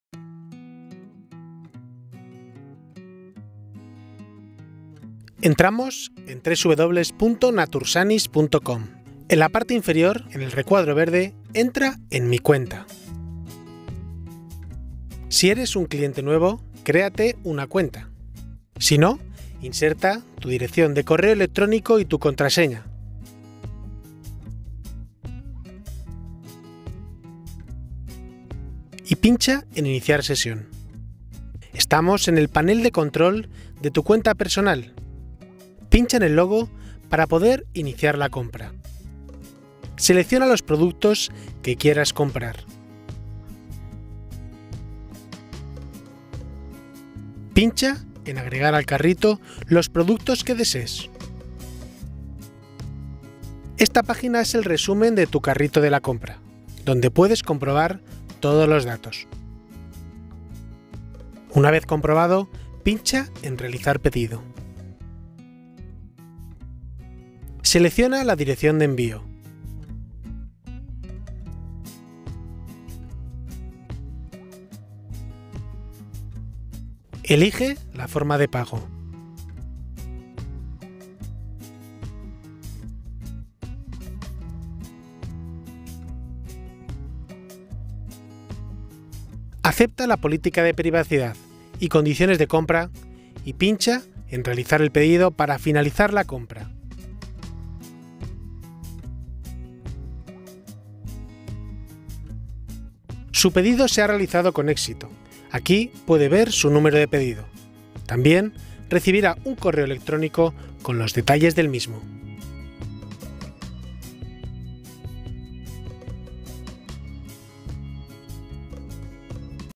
TUTORIAL:¿CÓMO COMPRAR EN NATURSANIX.COM?
Tutorial sencillo donde se explican los pasos para realizar una compra la web de salud natural.